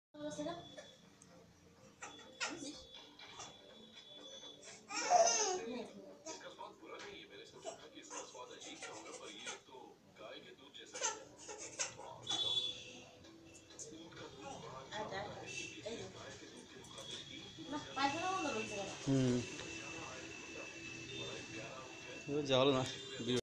Alivia's first step is for beloved mom Rina
Her mom is calling  Alivia to walk